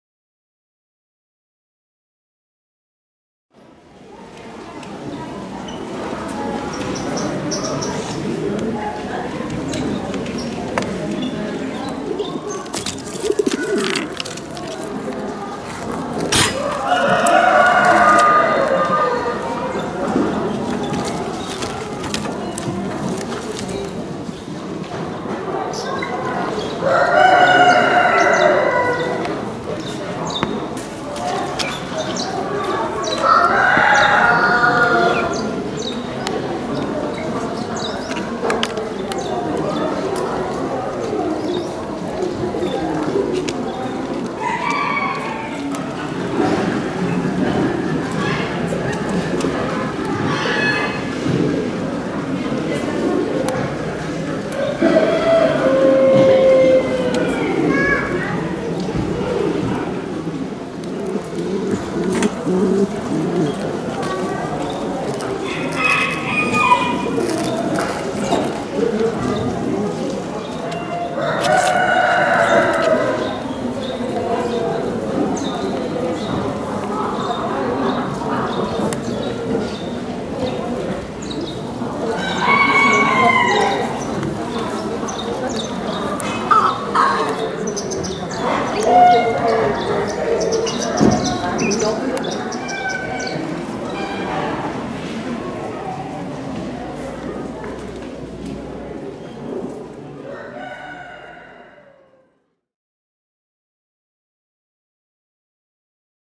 Pigeon Tambour Allemand à Coquille et Visière Jaune - عازف البوق الألماني الحمام
Exposition Nationale d'Aviculture de Brie-Comte-Robert 2021 - Organisation " Société Avicole Brie et Gâtinais ".
Double Crested German Trumpeter Pigeon - Deutsche Doppelkuppige Trommeltaube - Duitse Dubbelgekapte Trommelduif - Tambor Aleman de Doble Visera - Tamburo Tedesco a Doppio Ciuffo - Голубь трубач Немецкий - تاسعاً: الدمكش الألماني ذو التاجين